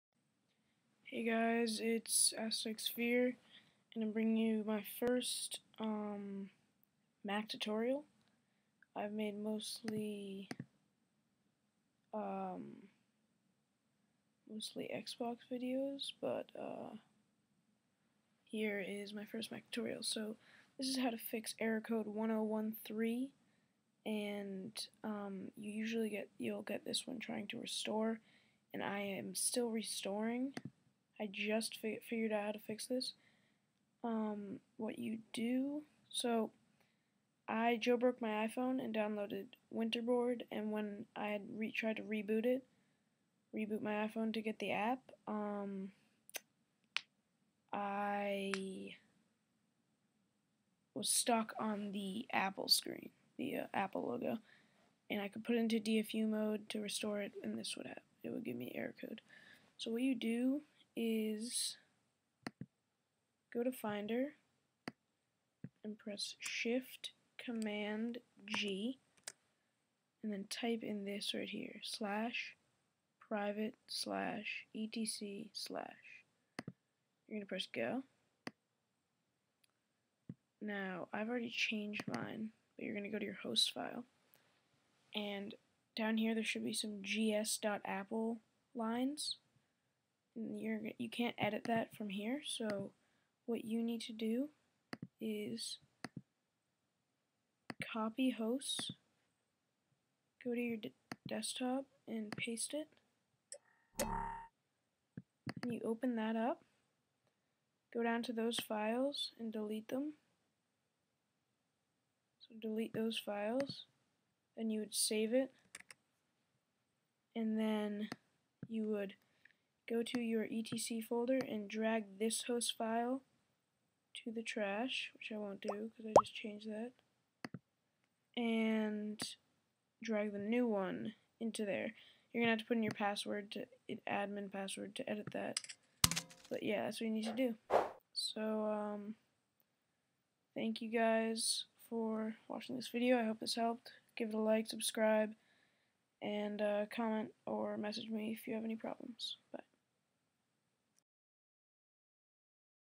How To: Fix Error 1013
this is how to fix error 1013 when trying to restore your apple device.

1)Open Finder

2)Command+Shift+G

3)Type "/private/etc/"

4)Copy Hosts file

5)Paste to desktop

6)Open File

7)Delete gs.apple lines at bottom

8) Move old hosts file to trash

9)Move New Hosts file from desktop to "etc"

Message me or comment for any help, Thanks